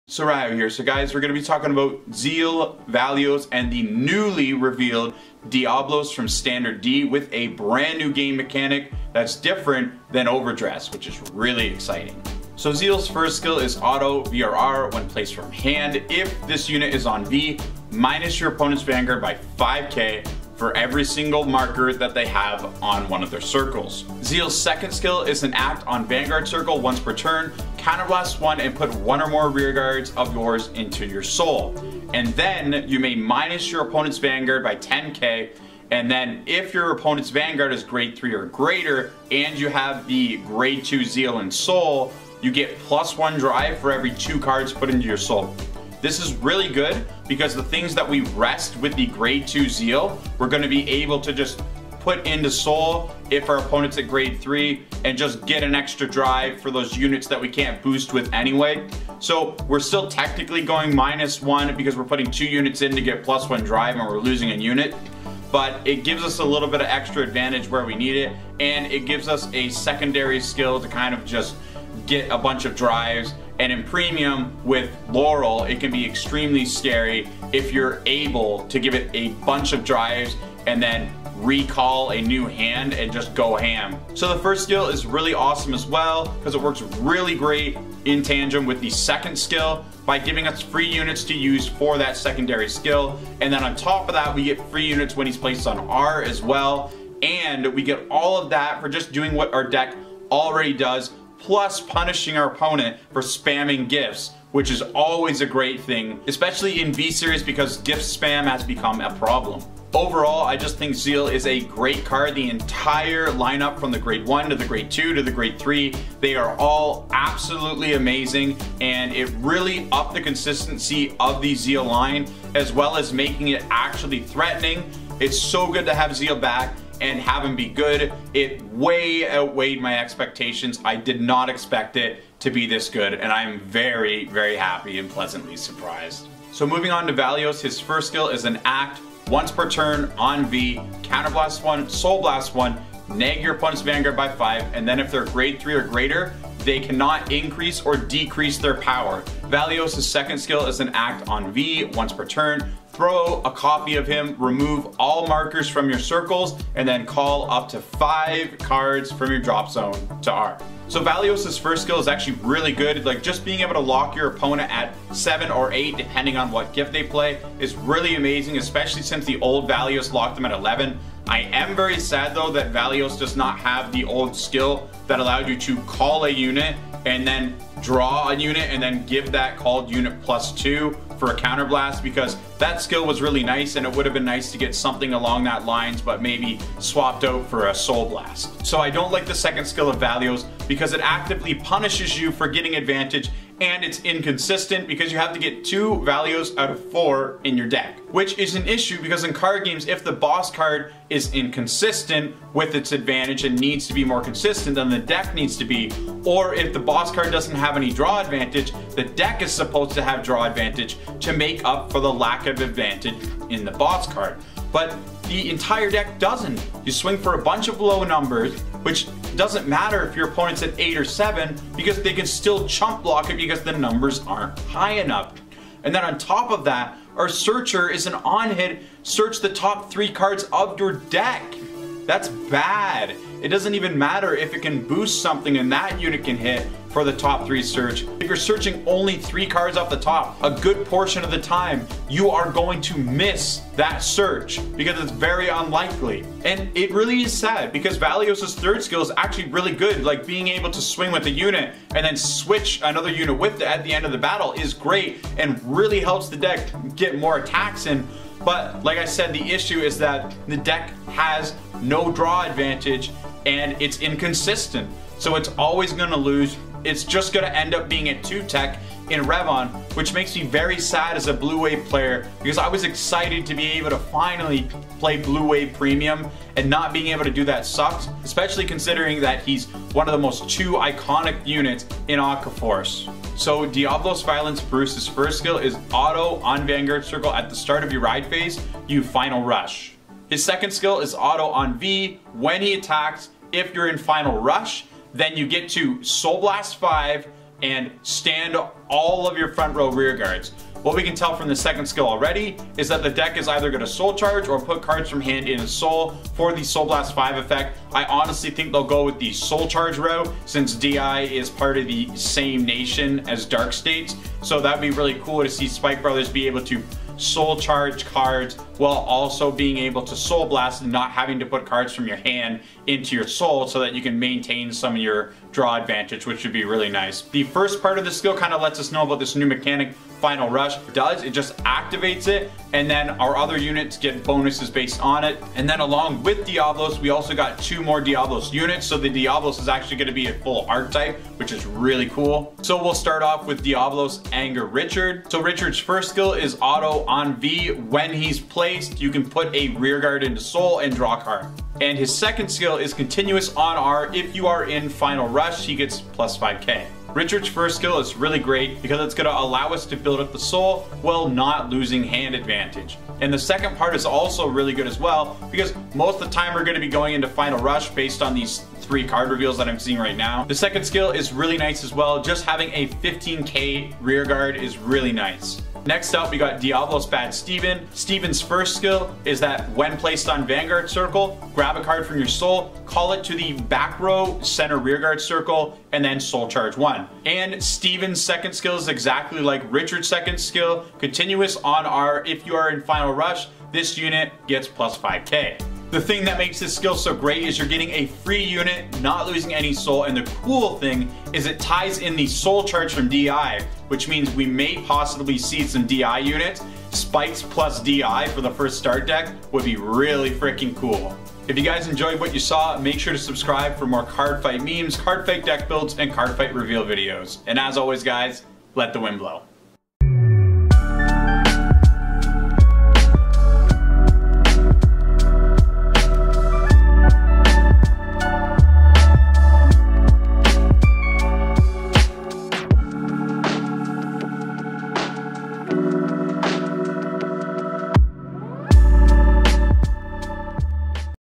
Zeal is back!!! + Bruce & friends reveal.
A reveal of the New Zeal, Valeos, & Diablos Violent Bruce & his friends.